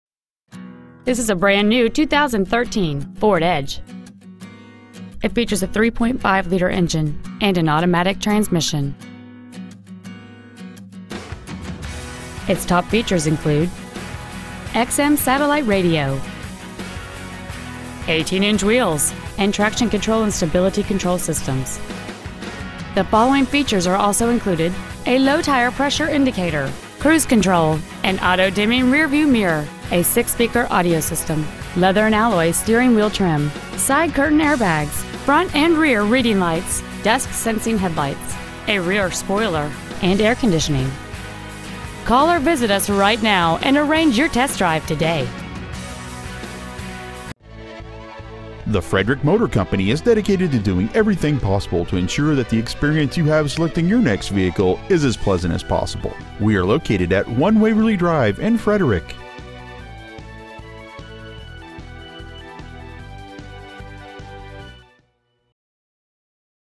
2013 FORD EDGE Frederick MD
We have been honored to serve the Frederick MD area , we promise that your experience at our dealership will exceed your expectations ! Year : 2013 Make : FORD Model : EDGE Engine : 3.5L V6 24V MPFI DOHC Trans . : 6-SPEED AUTOMATIC Exterior : RUBY RED METALLIC TINTED CLEARCOAT Interior : MEDIUM LIGHT STONE Stock : 37753 The Frederick Motor Company One Waverley Drive Frederick , MD 21702